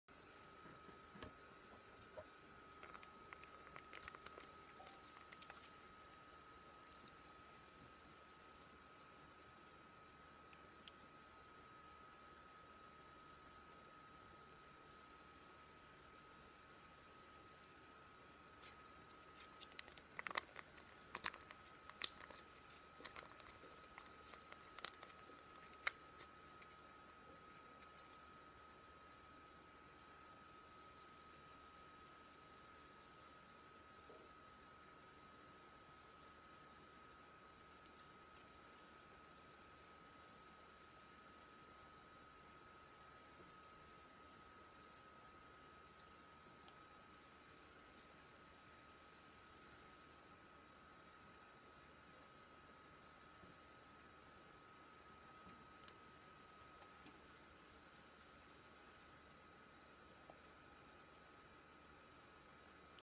Face Mask Skin Care Smooth Pearl Mask Asian Skin PART 1 How to maintain a smooth Facial Pimple Free
If you have any question of what Mask i have used just write a COMMENT and will answer you ASAP. God bless everyone and stay beautiful inside out!